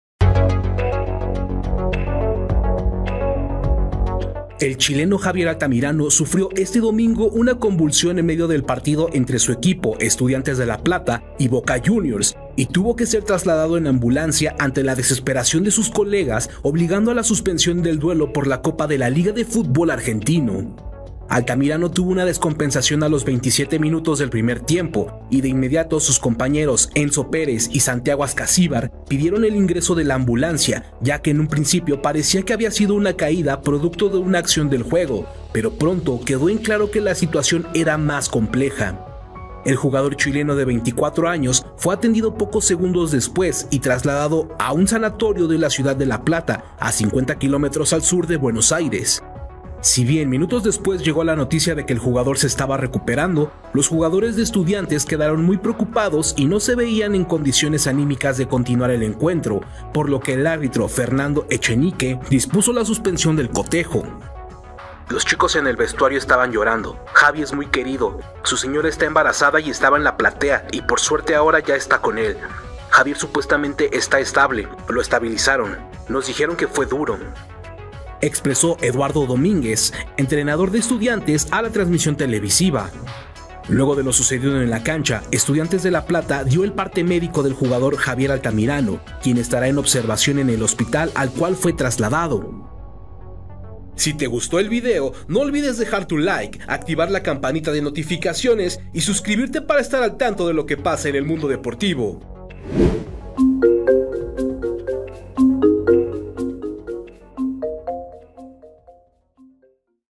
Javier Altamirano, jugador de Estudiantes La Plata, se convulsiona en partido ante Boca Juniors
El partido de la Copa de la Liga del futbol argentino tuvo que ser suspendido, pues los jugadores de Estudiantes La Plata no estaban en condiciones para continuar.